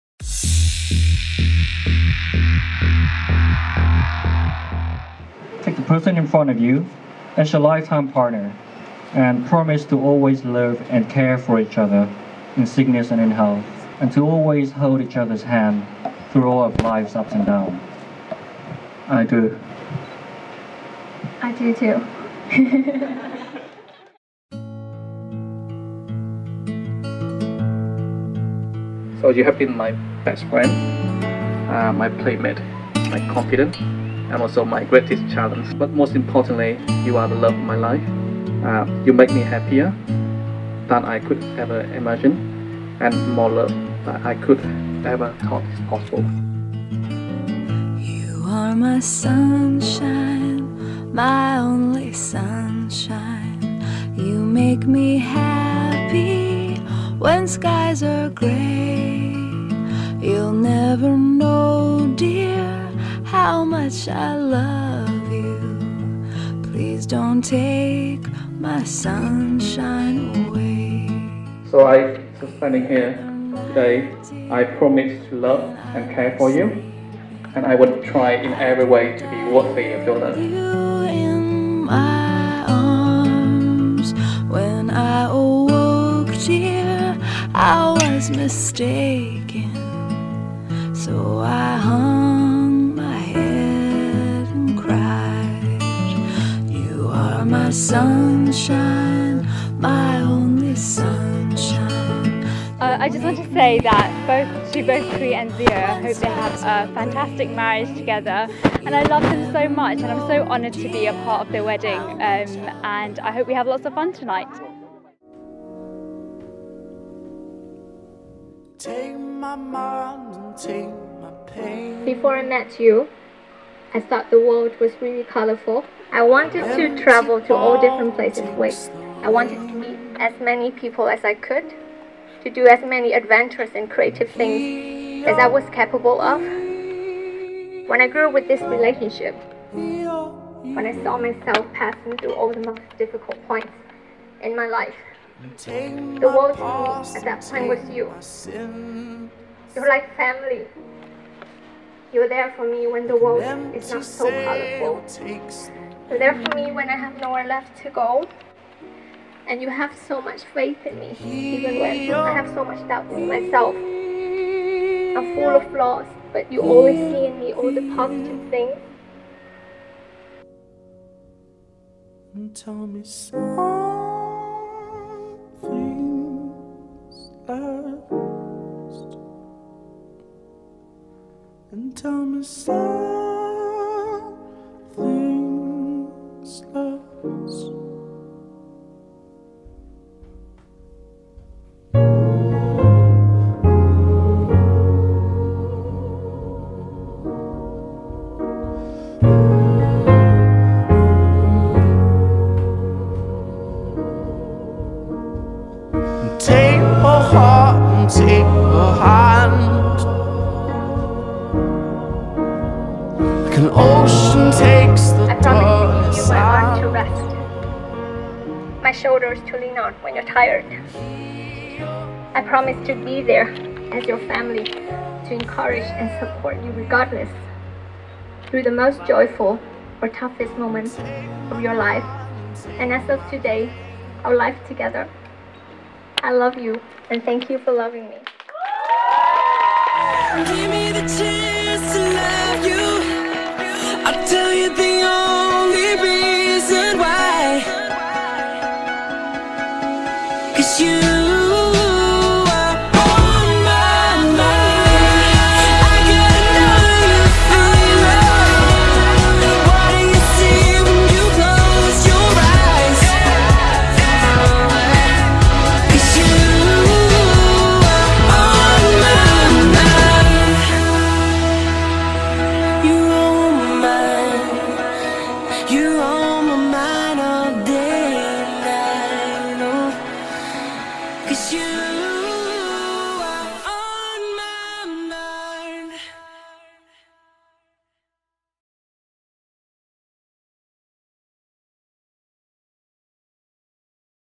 Ngoc Linh & Thanh Tri (Zèo Trí)| Wedding Highlights | WedinStyle | 2015
A garden themed wedding by WedinStyle
WedinStyle - The Stylish Wedding Planner of Vietnam
Địa chỉ: Tầng 2, 19 Phan Bội Châu, Hoàn Kiếm, Hà Nội